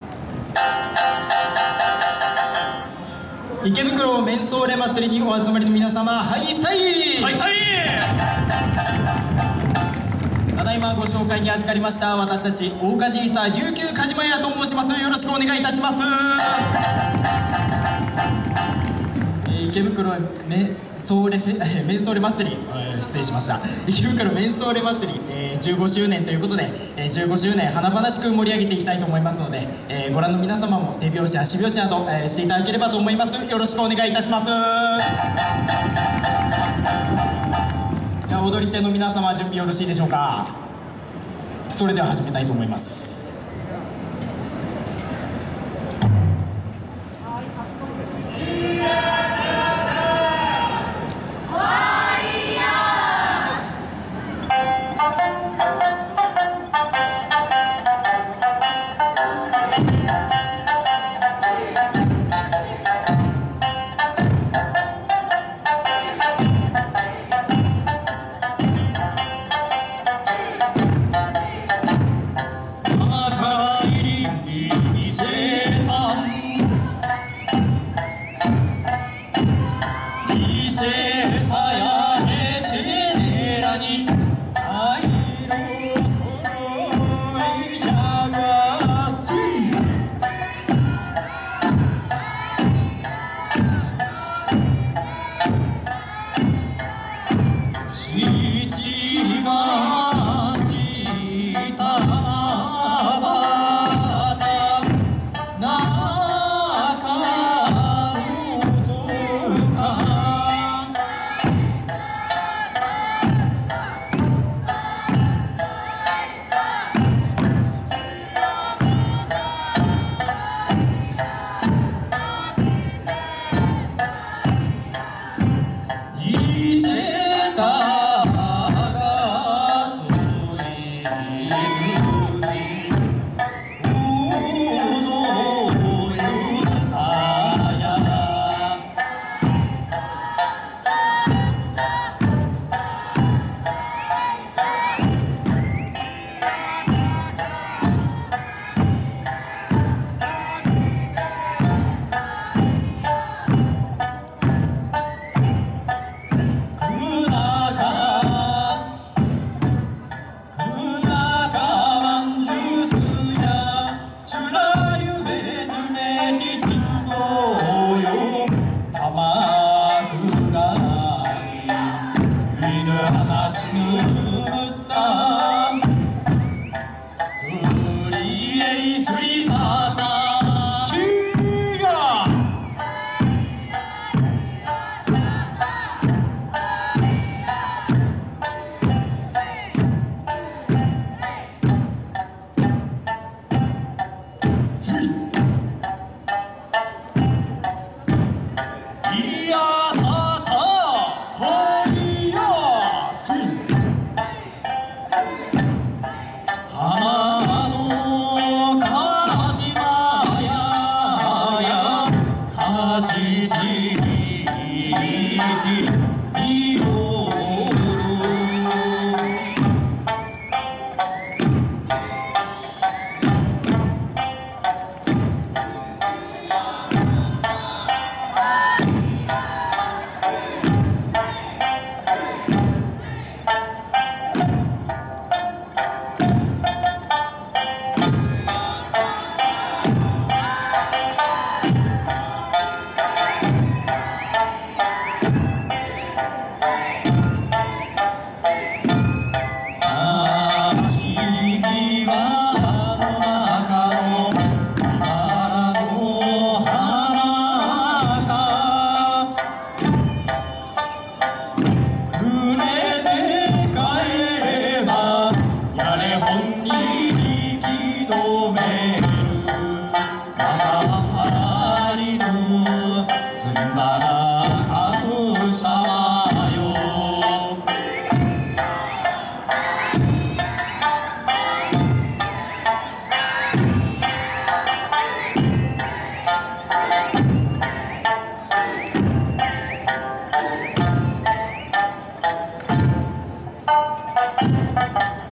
池袋めんそ～れ祭り 桜⾵エイサー琉球⾵車 3-1
24-05-26 池袋めんそ～れ祭りinサンシャイン60通り

凄い混み方の中での伸びあがっての撮影で、演舞が頭などで隠れています。